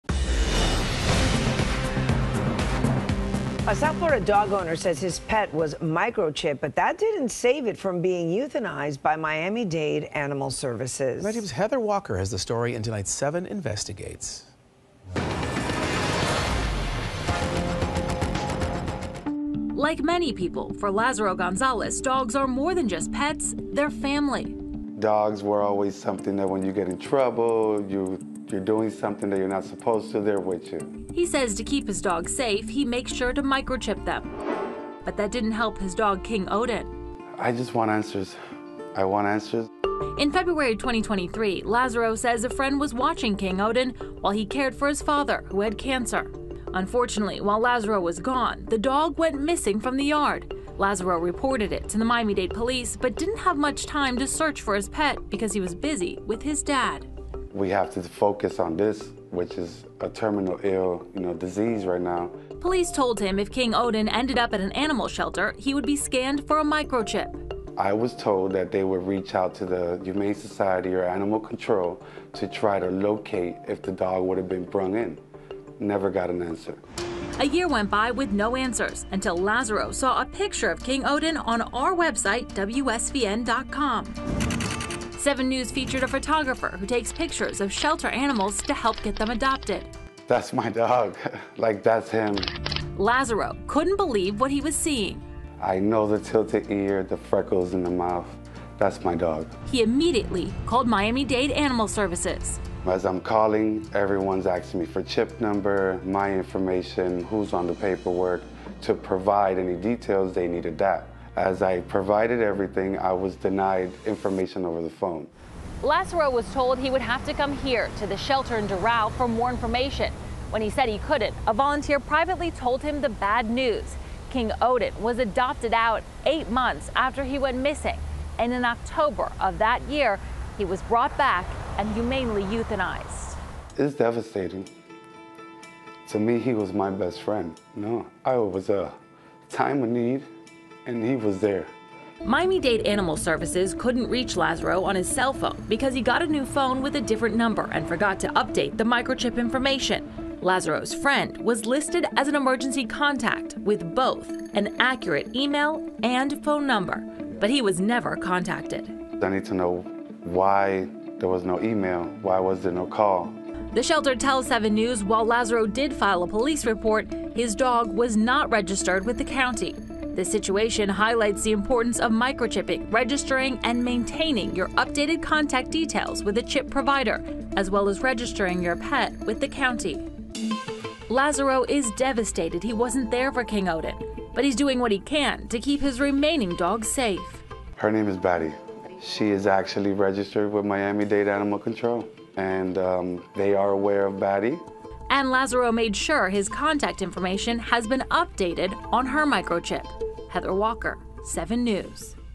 Owner says missing dog was microchipped, euthanized by Miami-Dade Animal Services
A South Florida dog owner said his pet was microchipped, but that didn’t save it from being euthanized by Miami-Dade Animal Services. 7’s Heather Walker investigates.